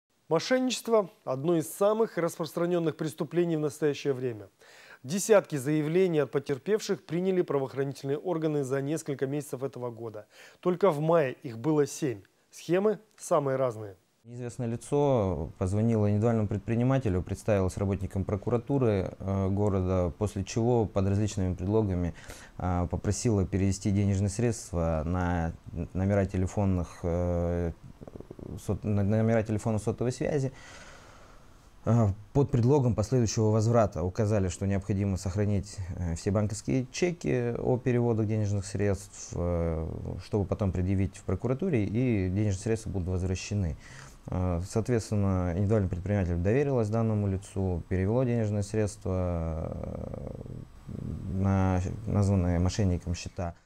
Осторожно! Мошенники!
Мошенничество одно из самых распространённых преступлений в настоящее время. Десятки заявлений от потерпевших приняли правоохранительные органы за несколько месяцев этого года. Только в мае их было семь. Схемы самые разные.